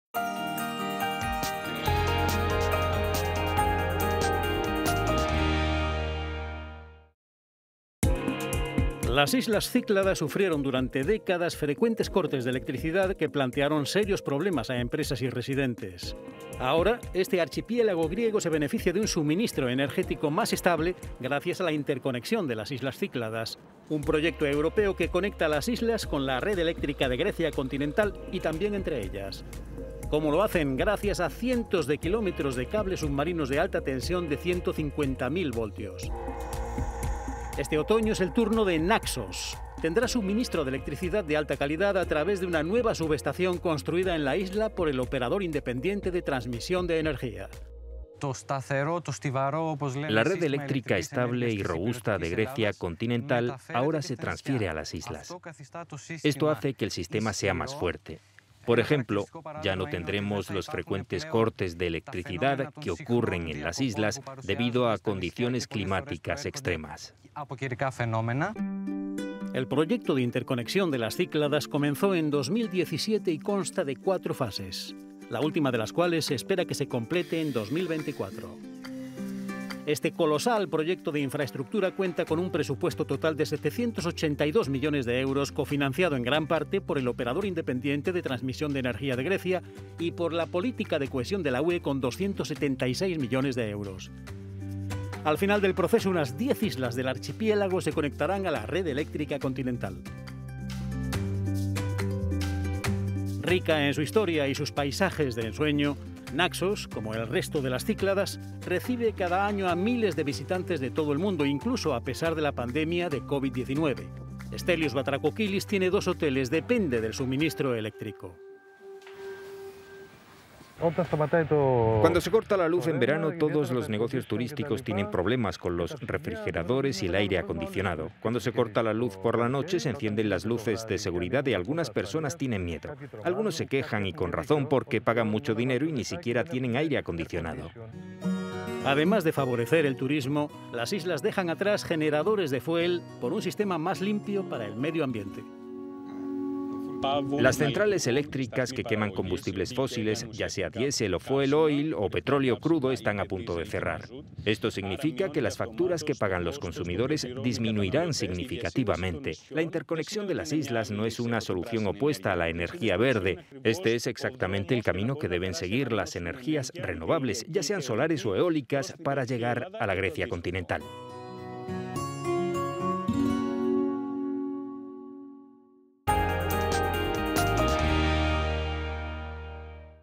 Las Cícladas conectadas a la red eléctrica continental
Tras años de cortes de luz, este archipiélago griego se beneficia de un suministro energético más estable gracias a la Interconexión de las Islas Cícladas, un proyecto europeo que conecta las islas con la red eléctrica de Grecia continental, y también entre ellas.